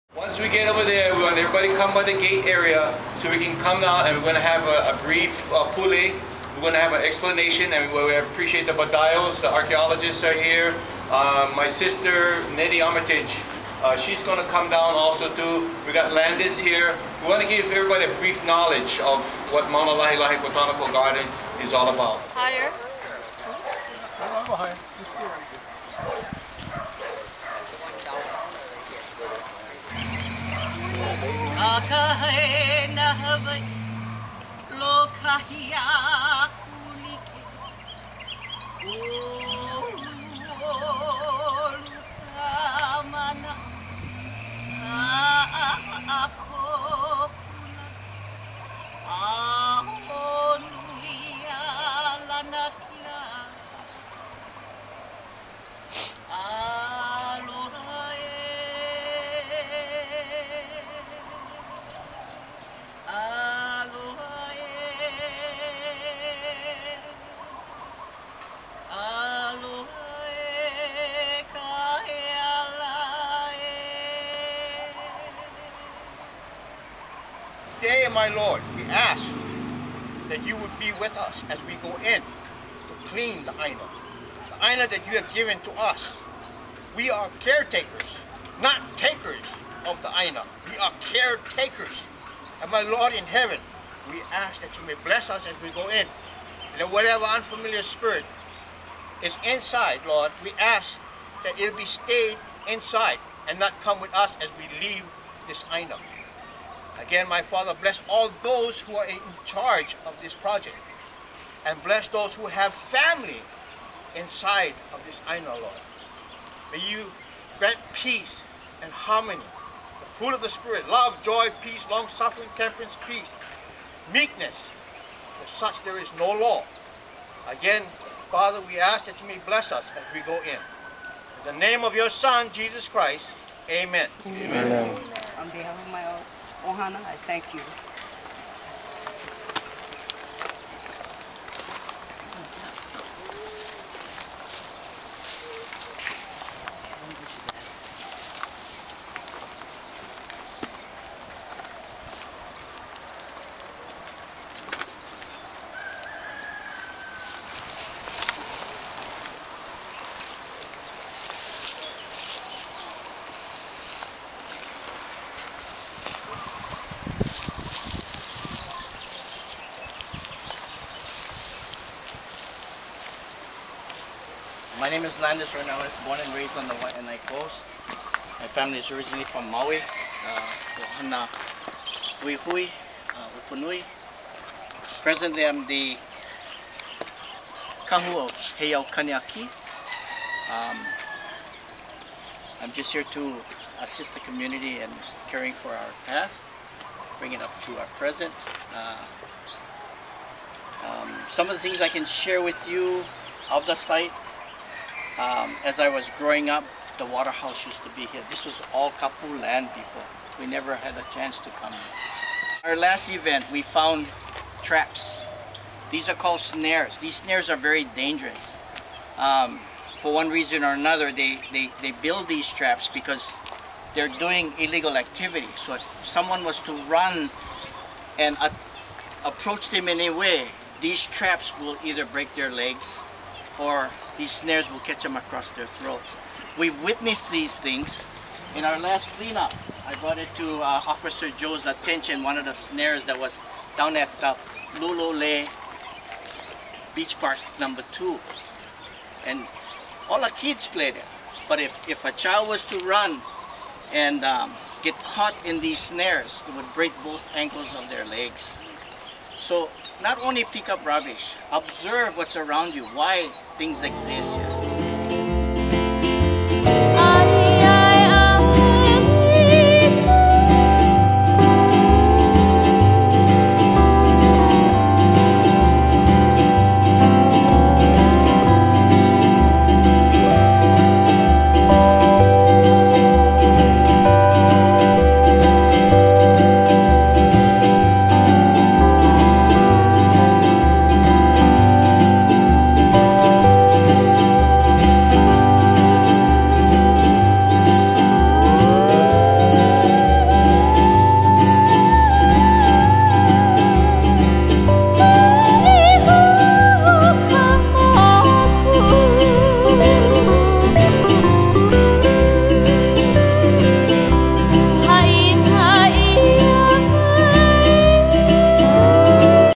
Makaha Hawaiian Civic Club Maunalahilahi Clean Up 4/7/12
MAUNALAHILAHI CLEAN UP APRIL 7, 2012 and afterwards volunteers got great food and fellowship to digest.